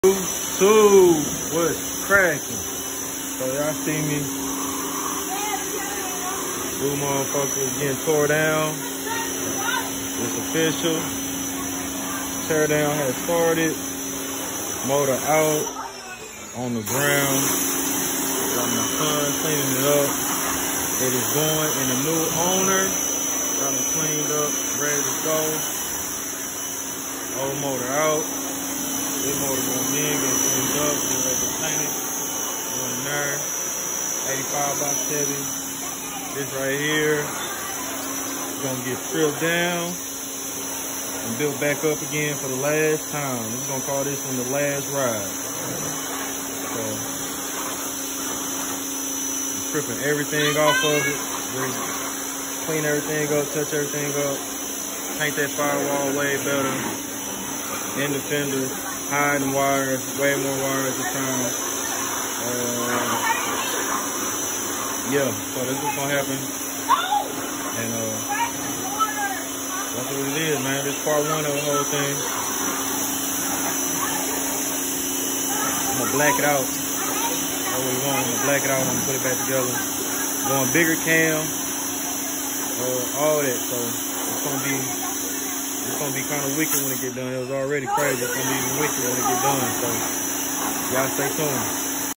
Tear down of the Monte Carlo SS
Sold the motor and swapping in 85 Caprice